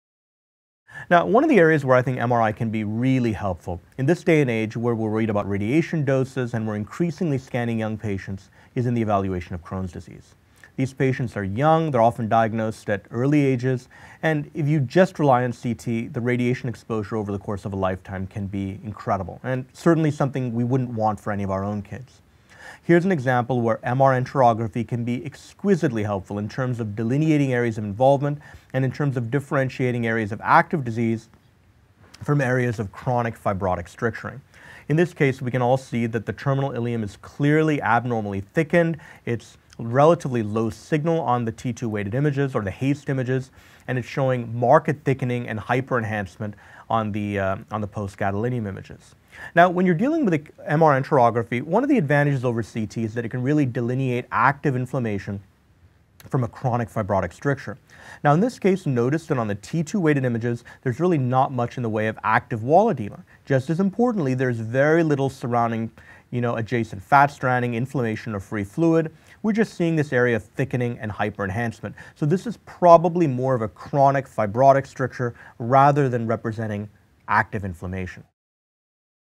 Crohn's disease
MRI images demonstrate extensive wall thickening and hyperenhancement of the terminal ileum, compatible with the patient's known Crohn's disease.